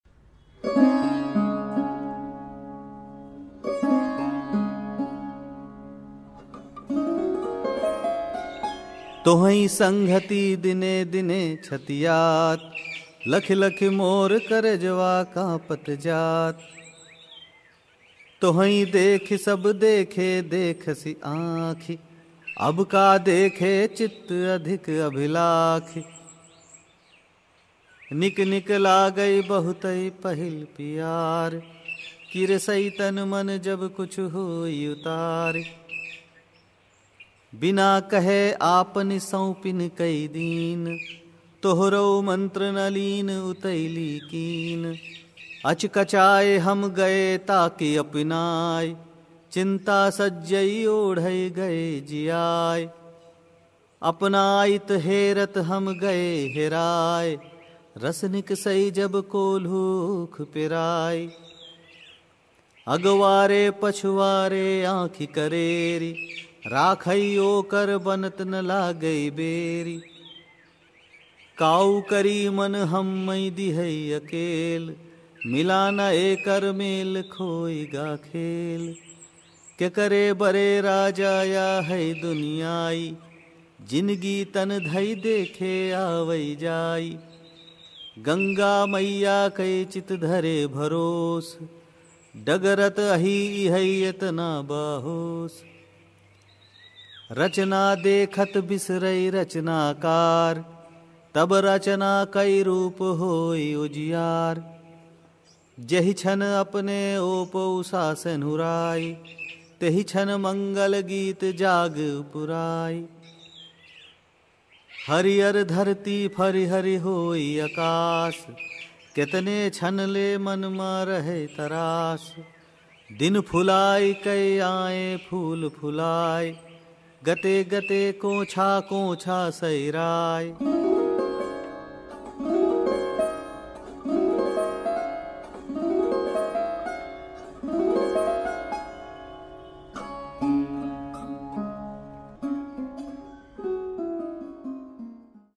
Trilochan | Poem 15 | Voice Pankaj Shrivastava
A 2008 recording.

Poem: Tohain sanghati

Poet: Trilochan, Voice: Pankaj Shrivastava

Produced by Irfan

Photo Pankhuri